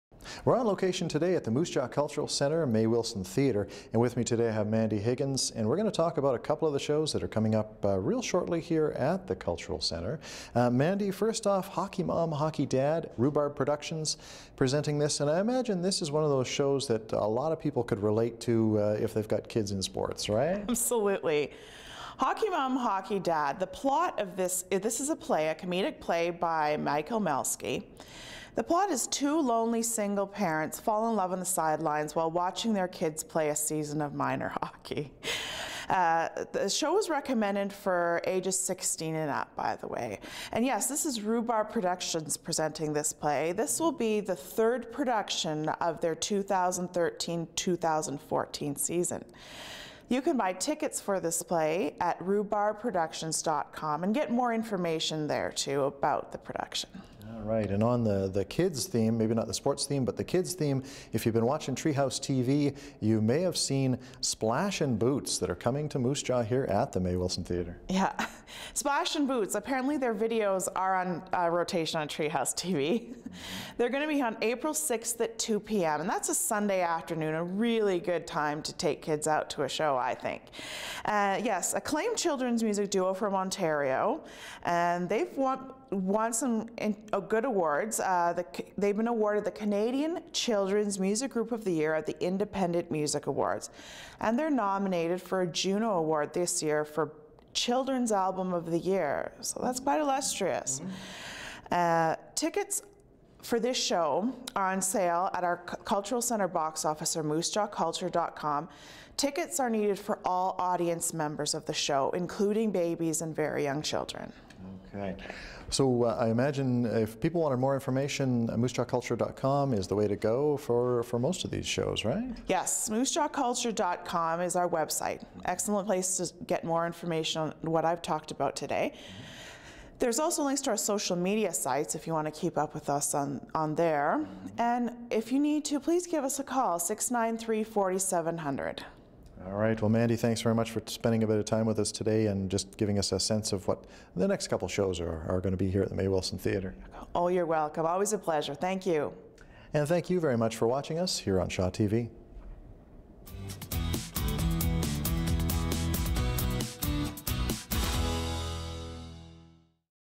Moose Jaw Cultural Centre shows April 3 and 6, 2014
Hockey Mom, Hockey Dad & Splash 'N Boots are just two shows coming up at the Mae Wilson Theatre April 3 & 6, 2014